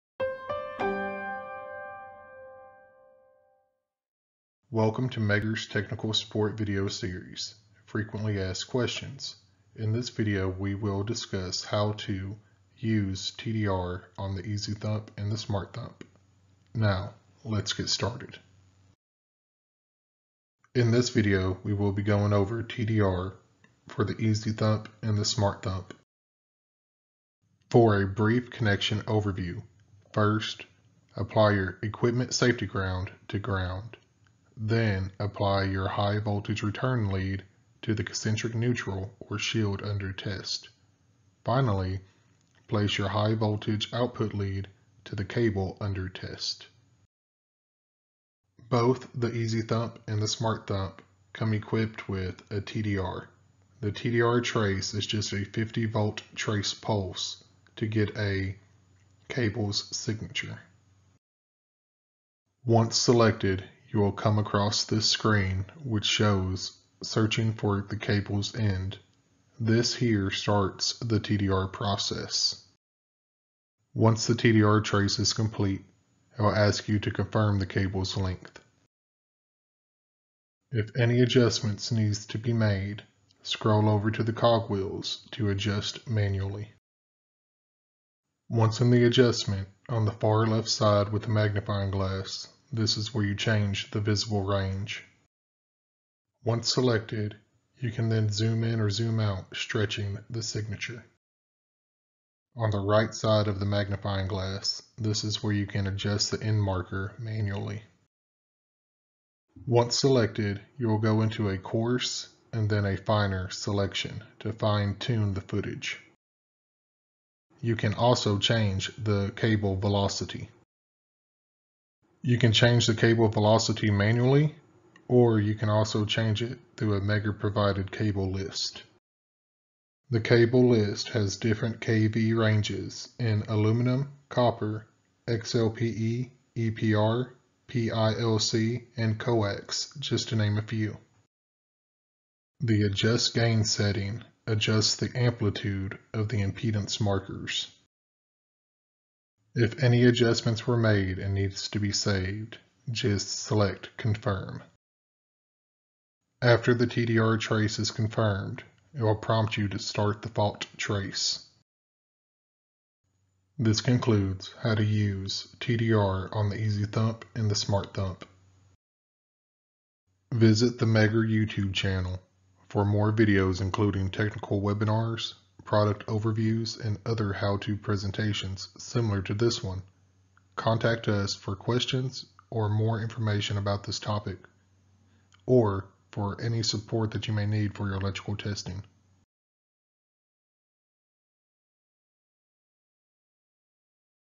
How to use Fault locating on EZ Thump 12 and Smart Thump 25
The EZ Thump and Smart Thump uses software that utilizes algorithms to determine the end of the cable. This video will discuss the guidelines on setting adjustments for TDR and connections on the EZ Thump and Smart Thump 25-30.